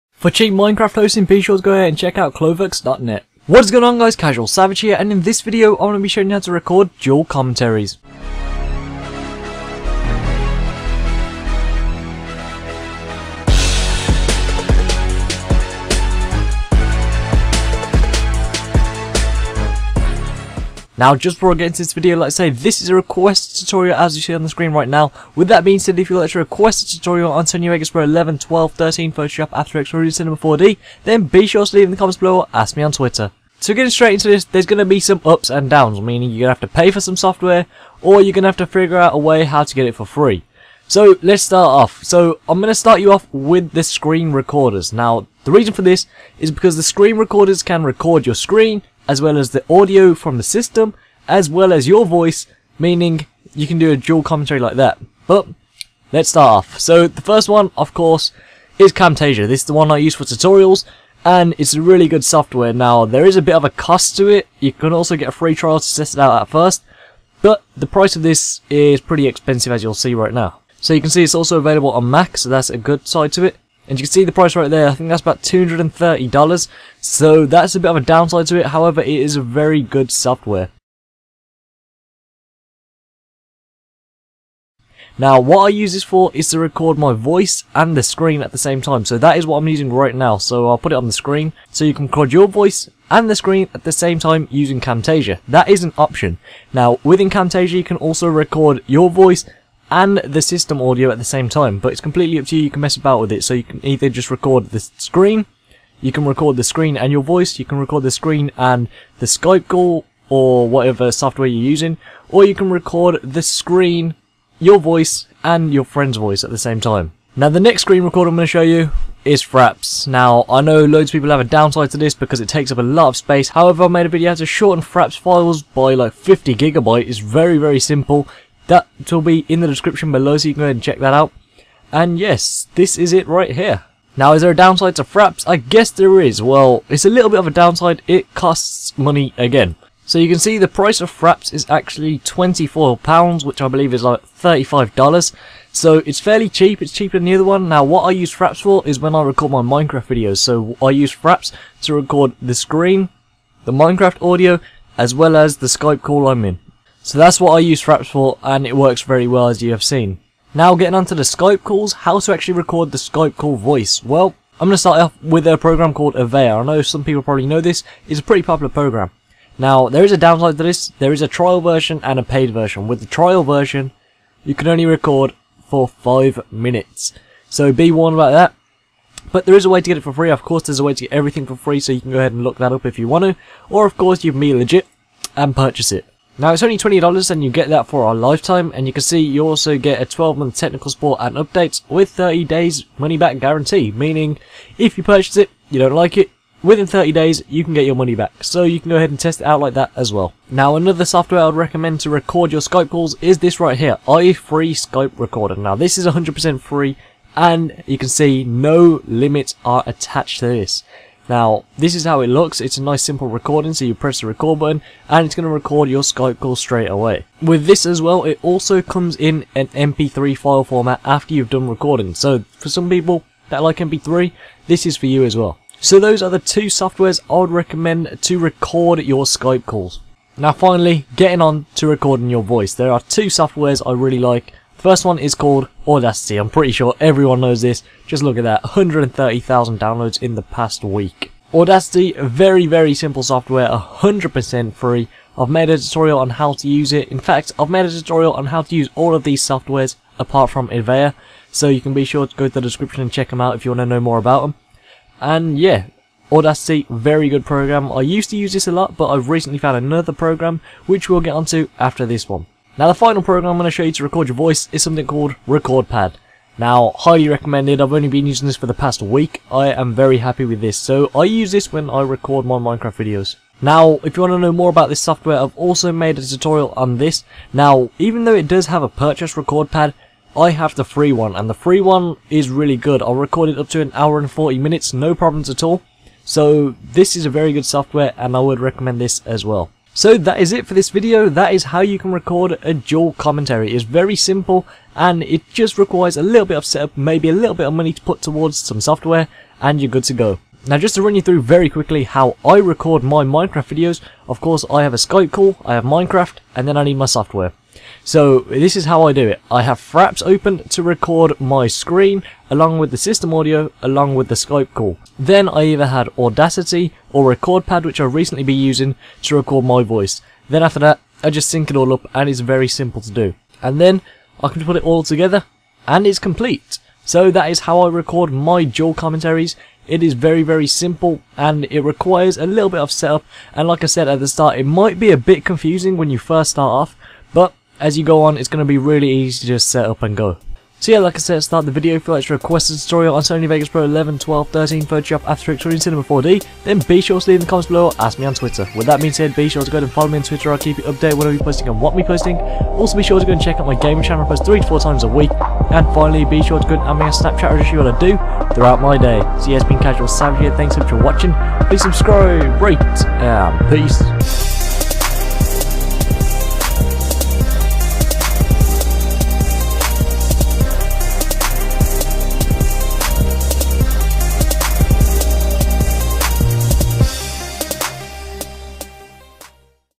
How To: Record Dual Commentary
How To: Record Dual Commentary! What's up, in this video I will be showing you how to record dual commentary, this is very simple to do and I will be showing different software's you're able to use, some are paid and some are free.

VOICE RECORDERS:
Audacity:

SKYPE CALL RECORDERS:
Evaer:

Server IP:
CSFactions.clovux.net

►Skype:
CasualSavage